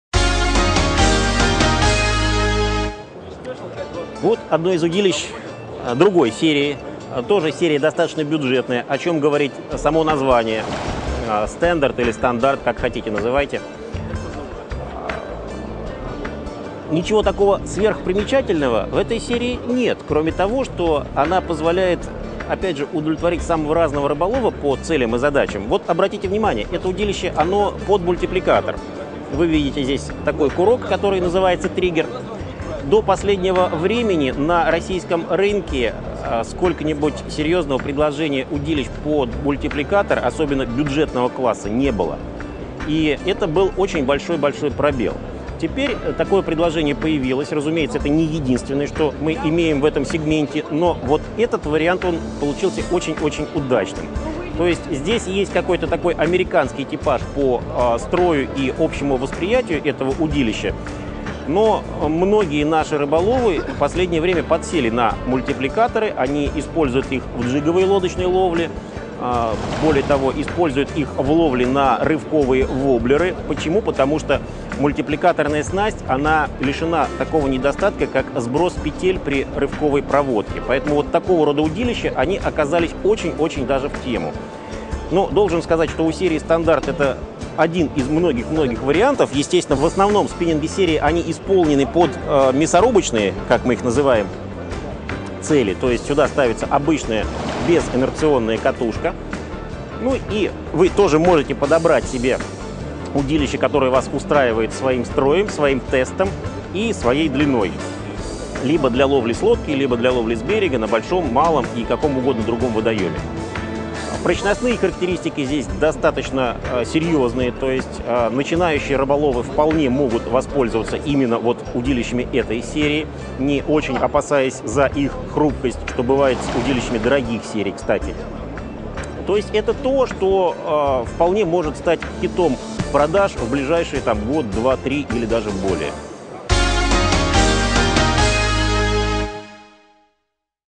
СПИННИНГ STANDARD - КОЛЛЕКЦИЯ NORSTREAM 2010.
К. КУЗЬМИН ПРЕДСТАВЛЯЕТ НОВУЮ МОДЕЛЬ СПИННИНГА NORSTREAM 2010 г.